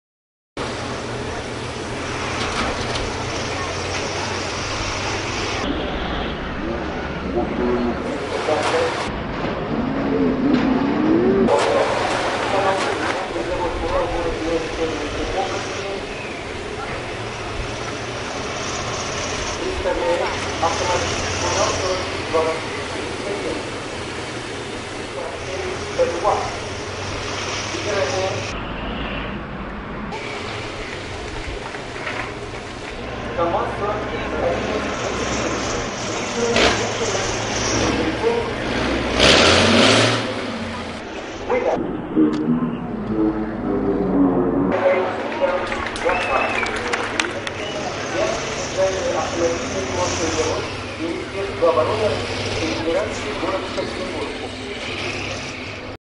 Russian Kamaz truck runs exhibition circuit
Russian Kamaz trucks at the arms expo in Nizhny Taghil. These trucks, along with Czech Tatras are the best in the world.